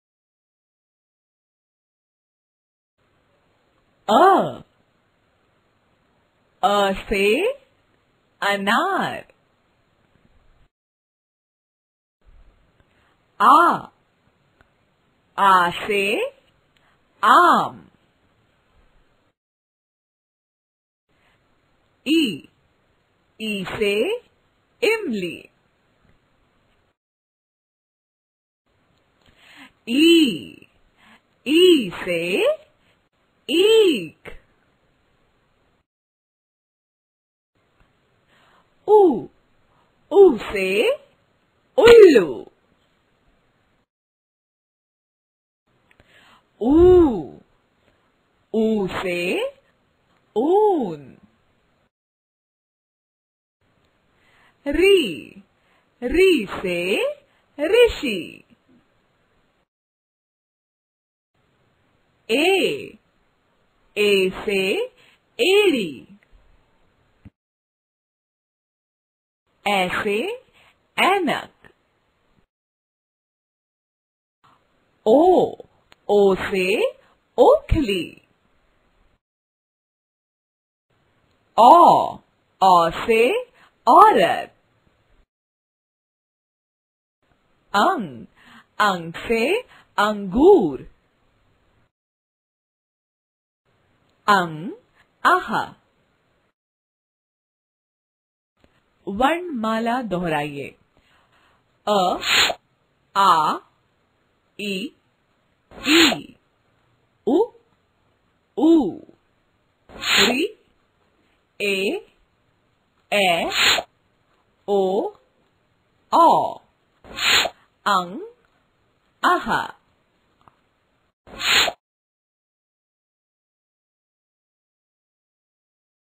हिंदी स्वर अ आ | Hindi Swar | Kids Learning
Learn Hind Swar A se aha, best video for learn at home, your kids can learn hindi for play group.

Preschool rhymes, kids Education Video, Kids Learning Video,
बच्चो के वीडियो, किड्स वीडियो, हिंदी कहानिया, प्रेरणा दायक कहानिया, हिंदी कवितायेँ,